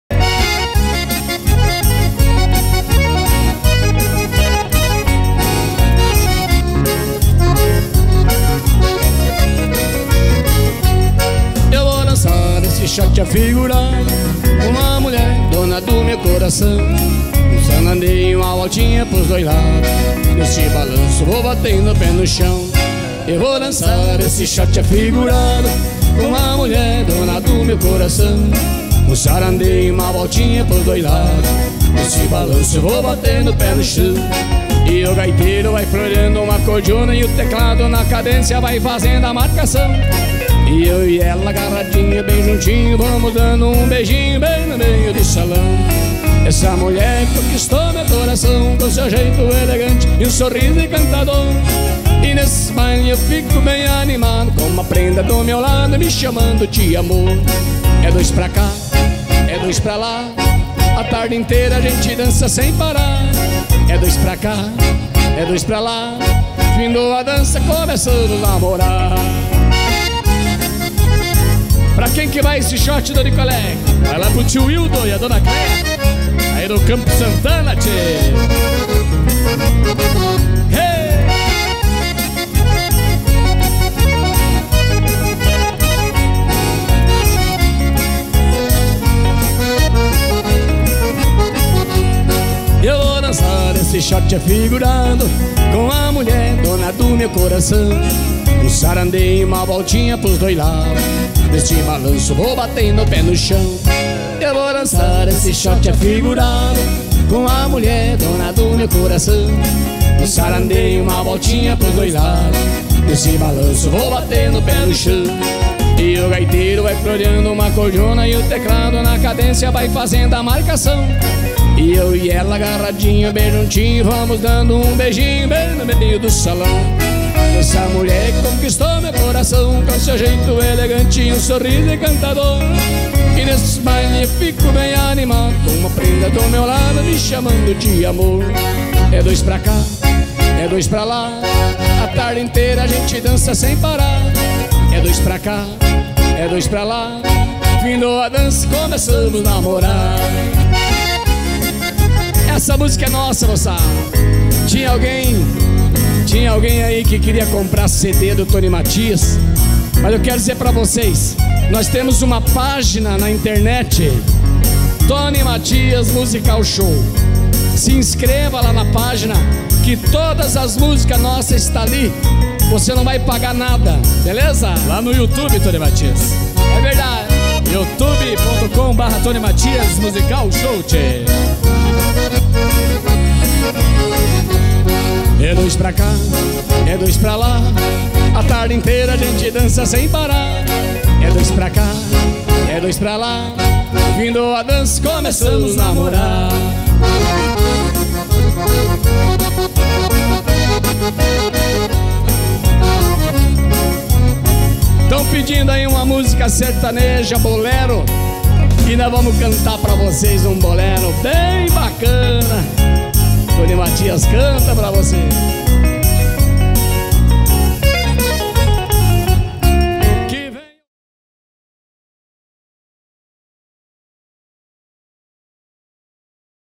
XOTE FIGURADO TONY MATHIAS AO VIVO EM CONTENDA-PR
ESSE XOTE FIGURADO, É UMA COMPOSIÇÃO DO TONY MATHIAS, ESPECIALMENTE PARA TOCAR NOS BAILES DA MELHOR IDADE, E FOI LANÇADA AO VIVO NO VIVA A MATURIDADE GENTE DOS 4 CANTOS EM CONTENDA-PR